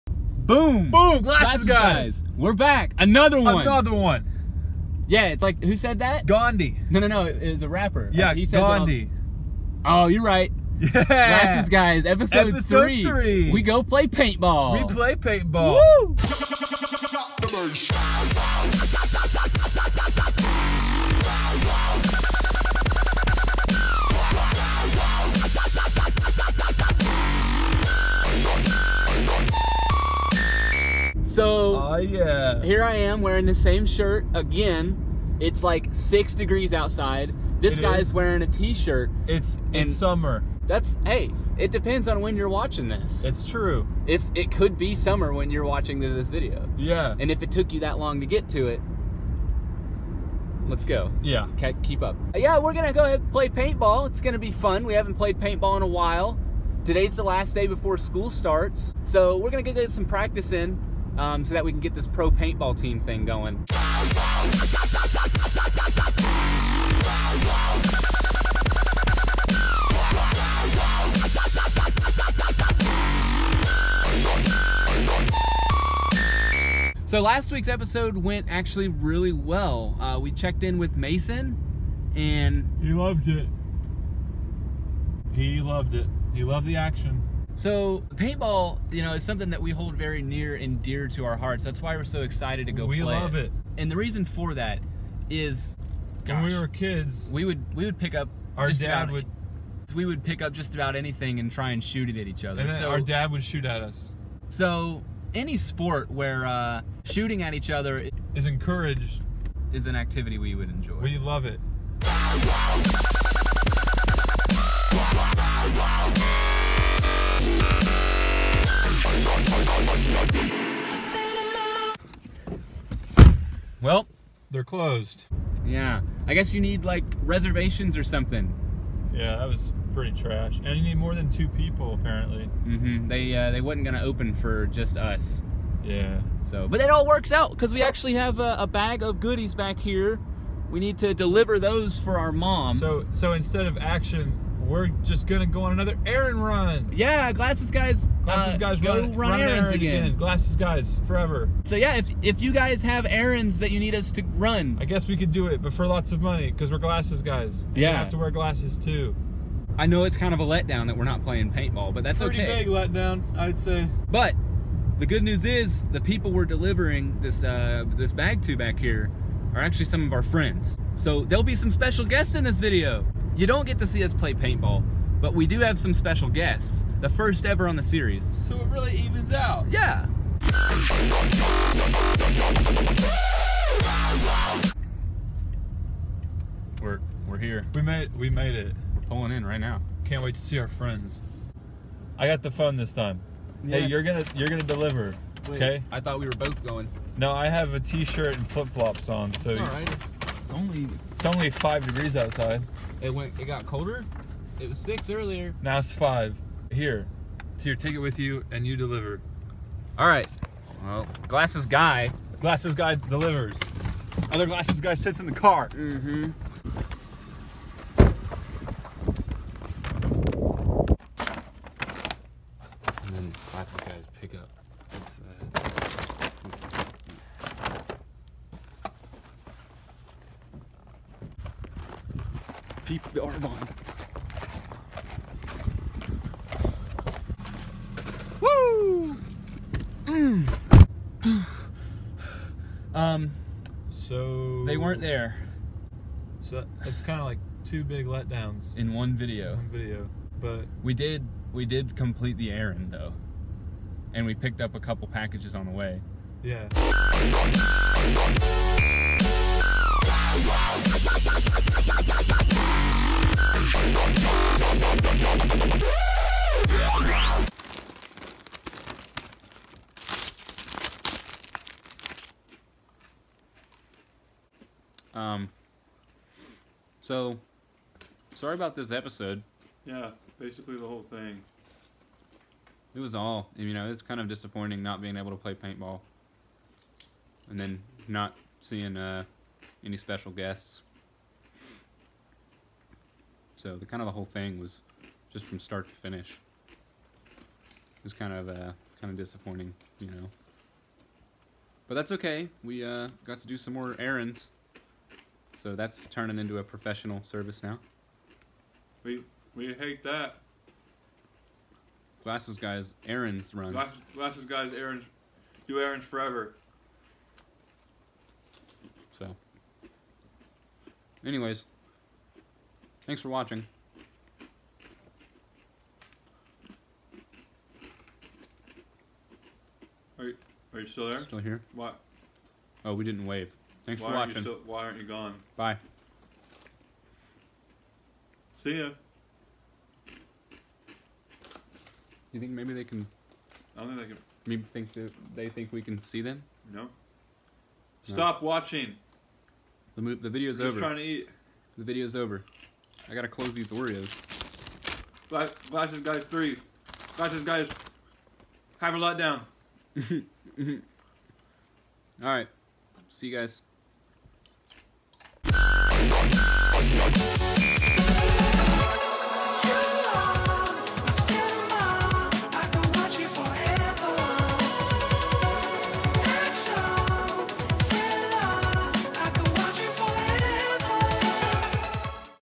GLASSES GUYS ep3
We decided that yet another episode was due, however, we didn’t think or plan ahead so here’s a big conglomeration of some clips we threw together after running around seymour for an hour. Enjoy. Or don’t.